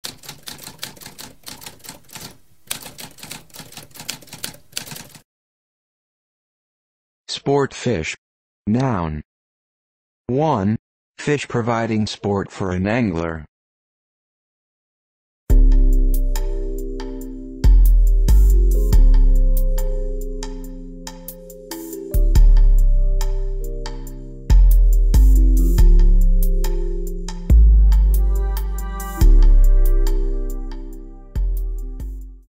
What does sport fish mean?
A spoken definition of sport fish.

Intro Sound:
Typewriter - Tamskp